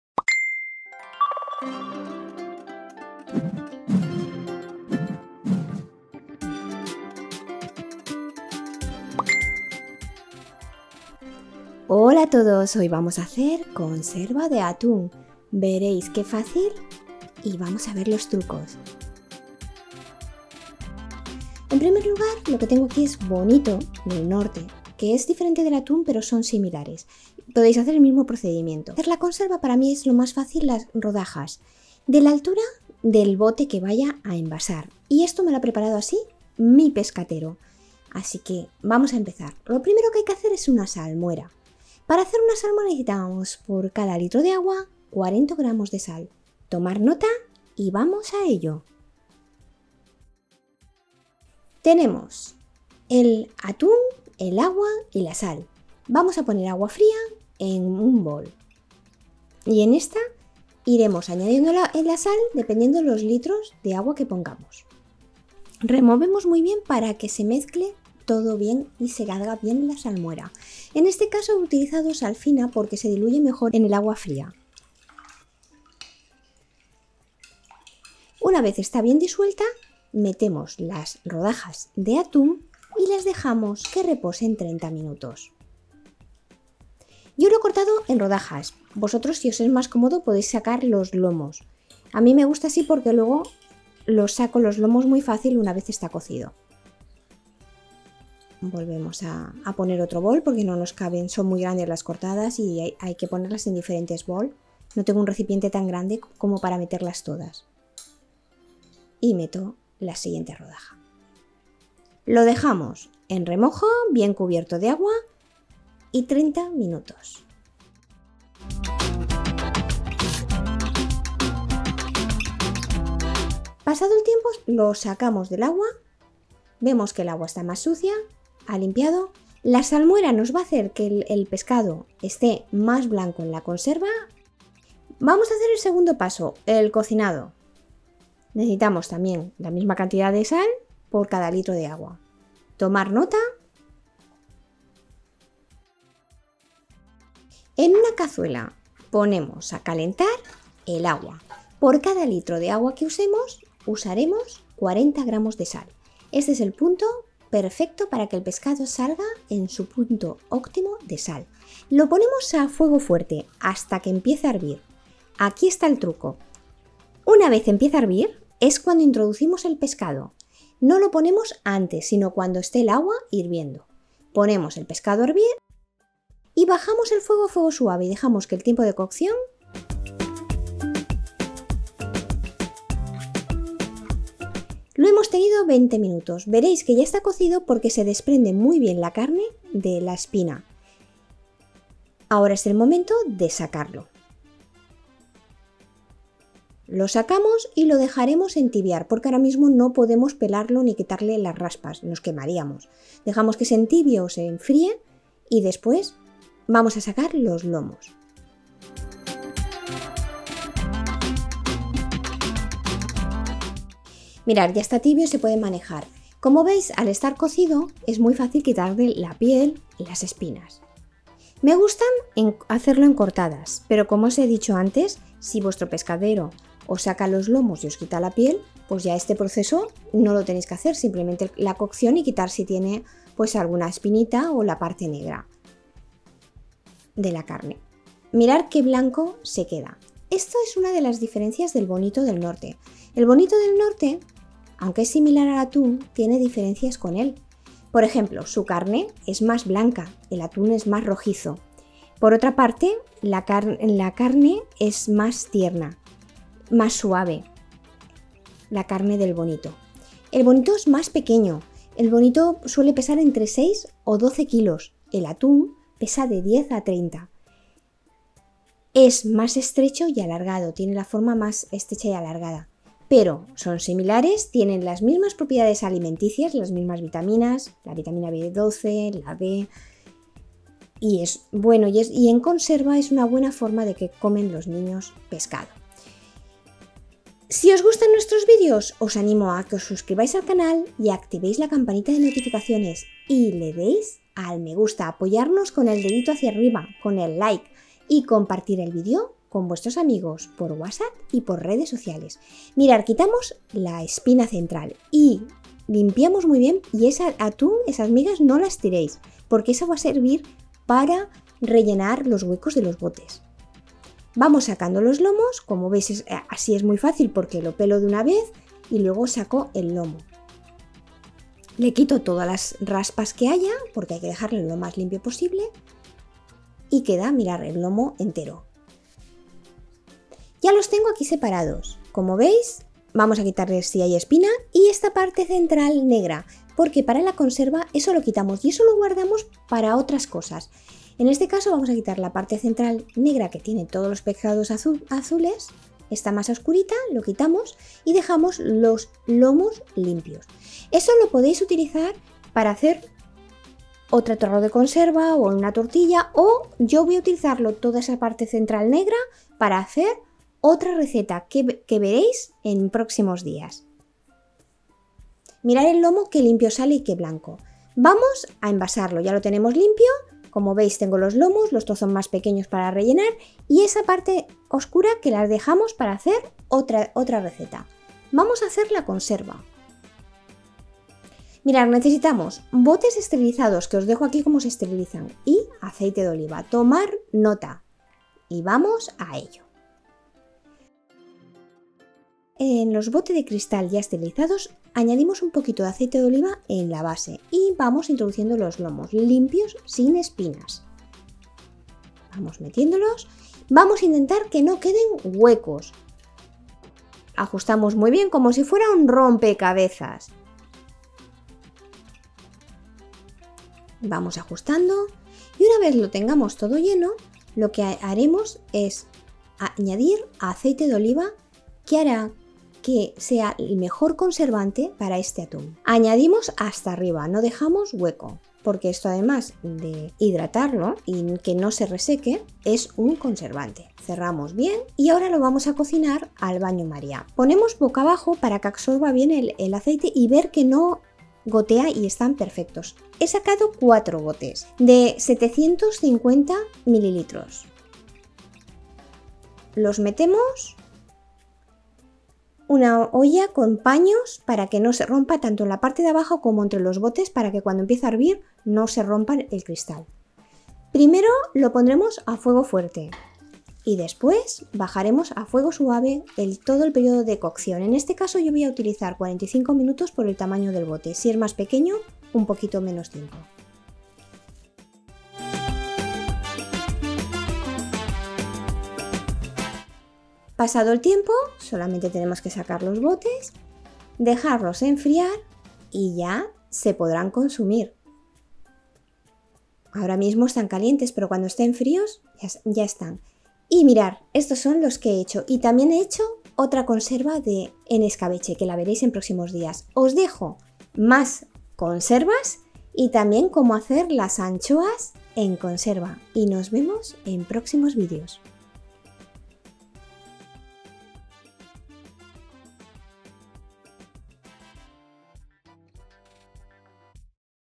ATÚN en CONSERVA con SÓLO 2 INGREDIENTES
Prepara atún en conserva en casa con sólo dos ingredientes.
En este video se muestra los mejores trucos para hacer la conserva casera de bonito del norte.
La receta nos muestra como puede hacer conserva de bonito de norte en casa y ahorrar dinero.
Disfruta de atún todo el año preparando esta conserva casera.

1º SALMUERA
1 litro de agua x 40 g de sal

2º COCCIÓN

1 litro de agua x 40 g de sal

3º CONSERVA